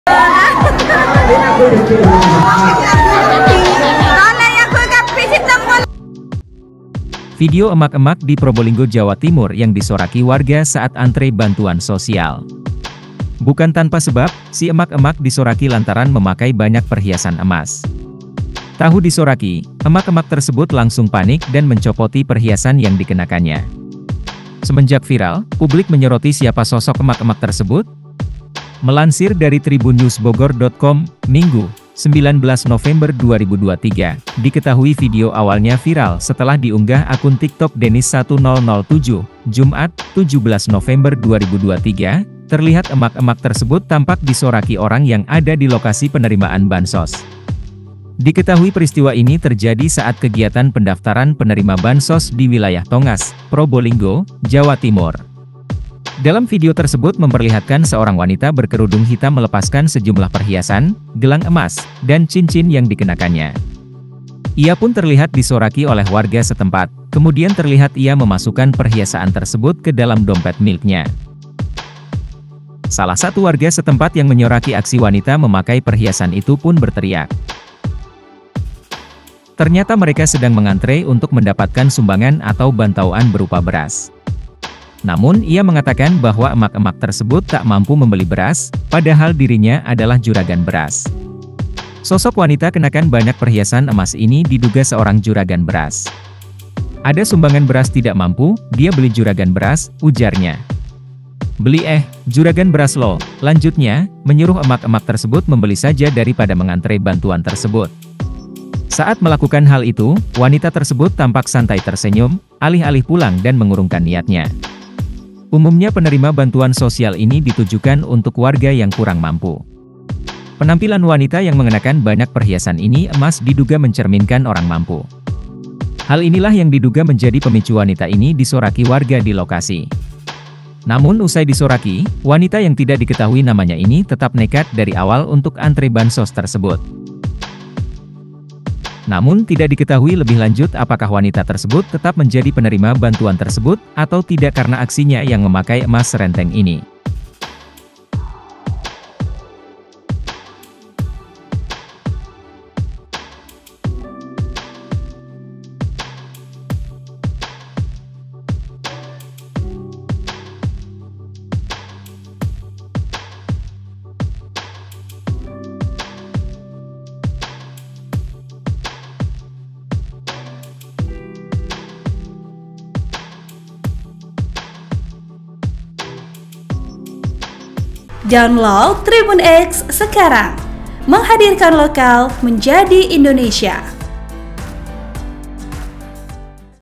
Emak-emak Disoraki Gegara Pakai Perhiasan Emas Banyak saat Antre Bansos
Video emak-emak di Probolinggo Jawa Timur yang disoraki warga saat antre bantuan sosial.

Bukan tanpa sebab, si emak emak disoraki lantaran memakai banyak perhiasan emas.

Tahu disoraki, emak-emak tersebut langsung panik dan mencopoti perhiasan yang dikenakannya.

Semenjak viral, publik menyoroti siapa sosok emak emak tersebut?

VP: Darwin